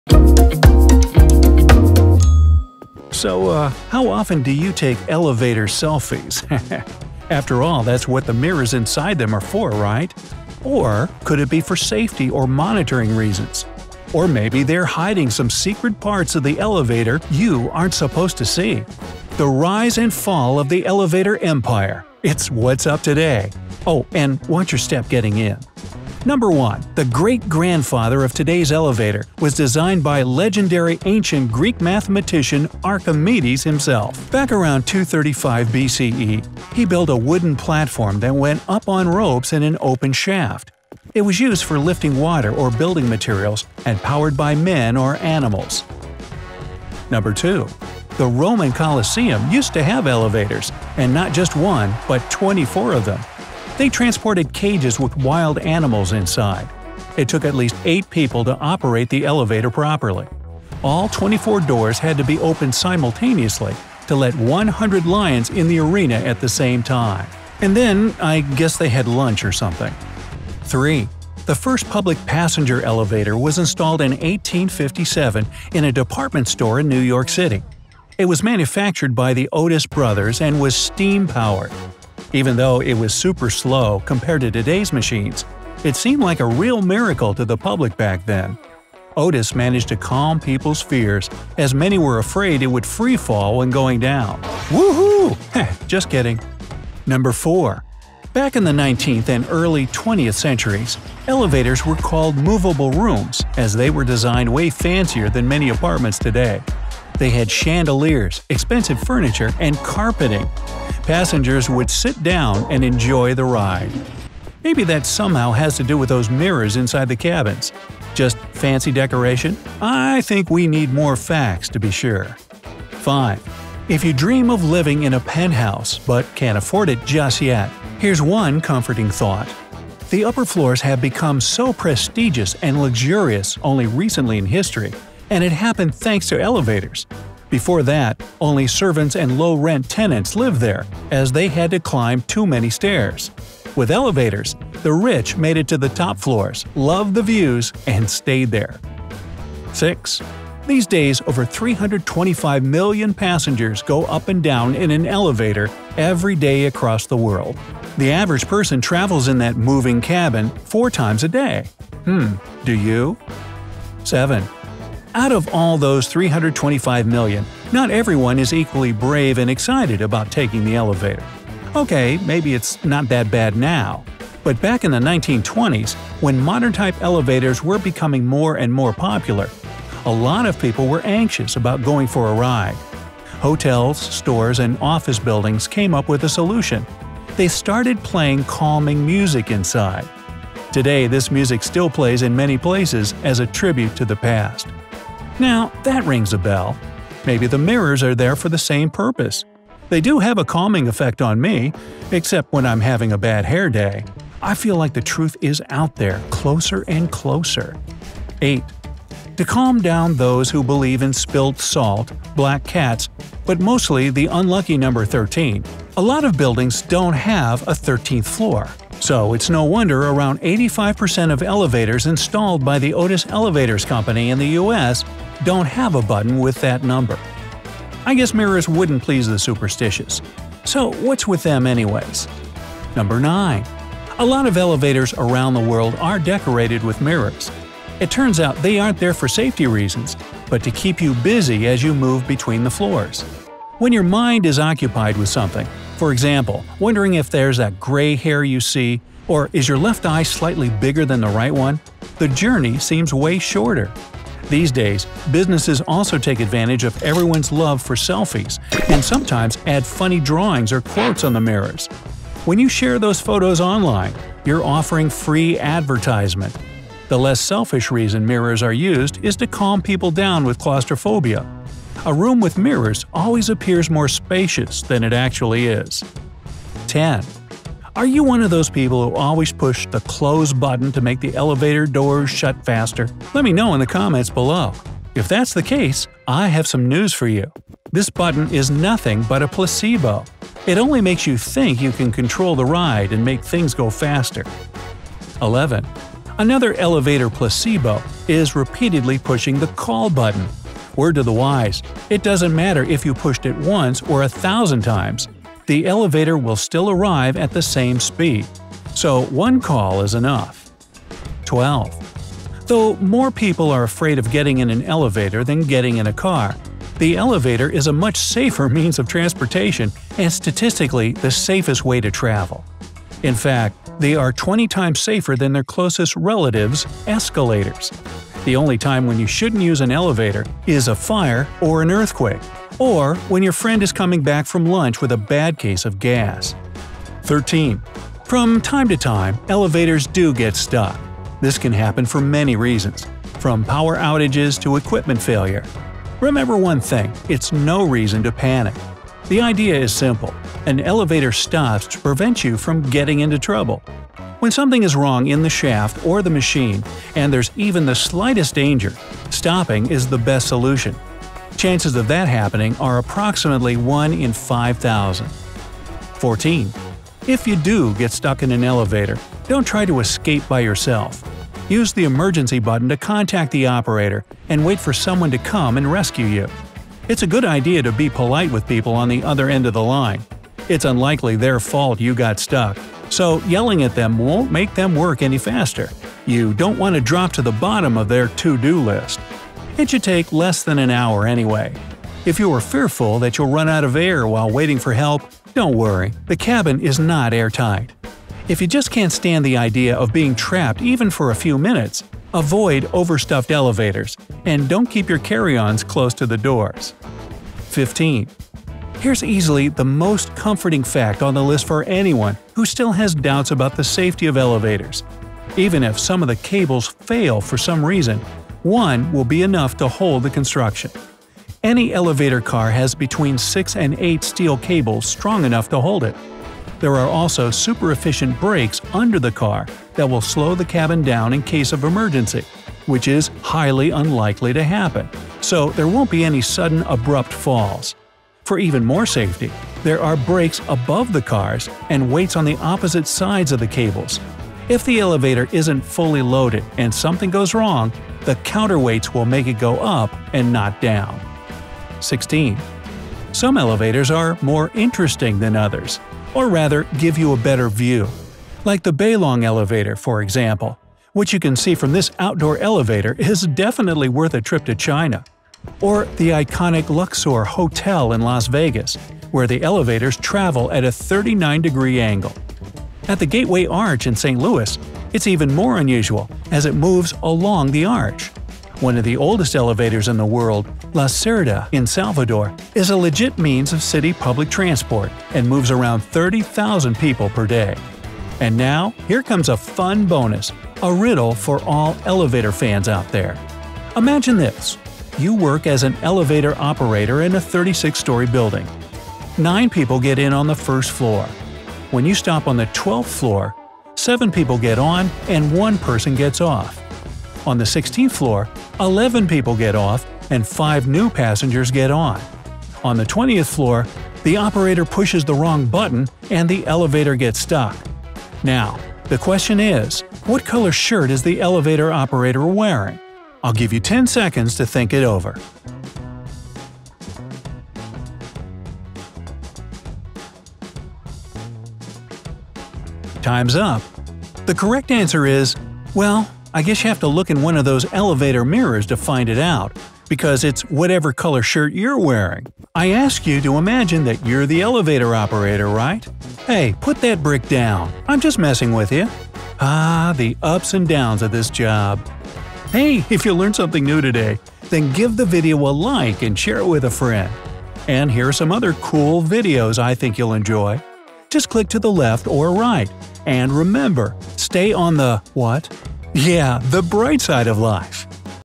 The True Purpose of Mirror in Elevators
How often do you take elevator selfies? After all, that’s what the mirrors inside them are for, right? Or could it be for safety and monitoring reasons? Or maybe they are hiding some secret parts of the elevator you aren’t supposed to see?

Back in the 1920’s when modern-type elevators were becoming more and more popular, a lot of people were anxious about going for a ride. Hotels, stores, and office buildings came up with a solution – they started playing calming music inside. So maybe the mirrors are there for the same purpose?

TIMESTAMPS:
Archimedes' elevator 0:26
The Roman Colosseum used to have elevators! 0:47
The first public passenger elevator 1:11
Buildings with no 13th floor 3:28
So what about mirrors? 3:53
Placebo button 🔴 4:37
What if you get stuck in an elevator 5:38
How safe elevators are 6:48
The coolest elevators in the world 7:33
A riddle for all elevator fans 🤓 8:11

SUMMARY:
- Back around 235 BCE, Archimedes built a wooden platform that went up on ropes in an open shaft.
- The Roman Colosseum used to have elevators and not just one but 24 of them!
- The first public passenger elevator was installed in 1857 in a department store in New York City.
-  Back in the 19th and early 20th centuries, elevators were called movable rooms as they were designed way fancier than many apartments today.
- The upper floors have become so prestigious and luxurious only recently in history, and it happened thanks to elevators.
- These days, over 325 million passengers go up and down in an elevator every day across the world.
- Mirrors are used to calm down people with claustrophobia. A room with mirrors always appears more spacious than it actually is.
- Are you one of those people who always push the “close” button to make the elevator doors shut faster? This button is nothing but a placebo.
- Though more people are afraid of getting in an elevator than getting in a car, the elevator is a much safer means of transportation and statistically the safest way to travel!
- An elevator stops to prevent you from getting into trouble. When something is wrong in the shaft or the machine, and there’s even the slightest danger, stopping is the best solution.
- If you do get stuck in an elevator, don’t try to escape by yourself. Use the emergency button to contact the operator and wait for someone to come and rescue you.
- Even if some of the cables fail for some reason, one will be enough to hold the construction!